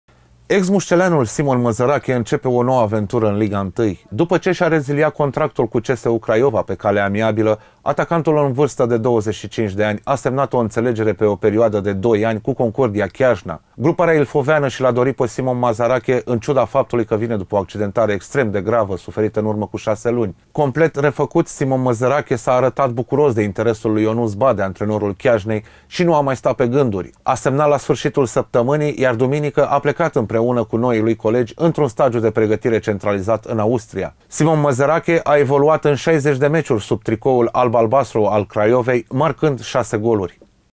stire02 07 2018 Simon Măzărache, la Concordia Chiajna
Simon Măzărcahe şi-a reziliat contractul cu CSU Craiova şi a semnat o înţelegere pe două sezoane cu Concordia Chiajna. Musceleanul a fost prezentat oficial de clubul ilfovean şi duminică, a plecat cu echipa în cantonament, în Austria.